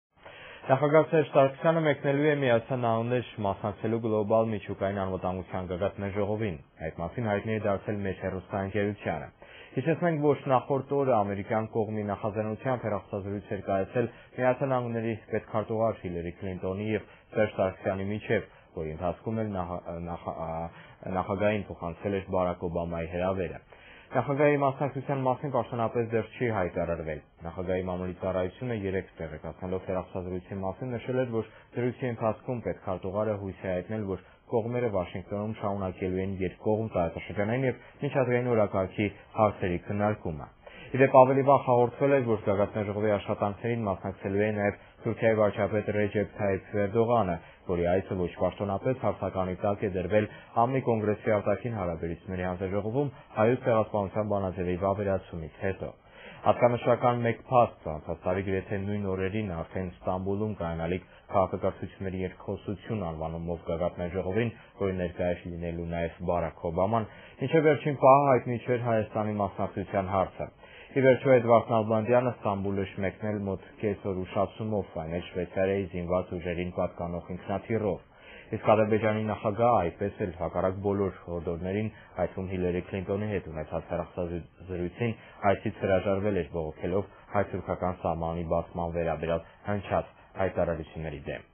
Սերժ Սարգսյանն ընդունել է Օբամայի հրավերը
ԱՄՆ պետքարտուղար Հիլարի Քլինթոնի փոխանցմամբ` Բարաք Օբաման Նախագահ Սերժ Սարգսյանին հրավիրում է Վաշինգտոն` մասնակցելու Գլոբալ միջուկային անվտանգության գագաթնաժողովին: Ըստ մեր ունեցած տեղեկության` հրավիրված է նաեւ Թուրքիայի վարչապետը: Մեր երկրի Նախագահն ընդունել է Բարաք Օբամայի հրավերը: Ռեջեփ Թայիփ Էրդողանի այցը ոչ պաշտոնական հարցականի տակ է: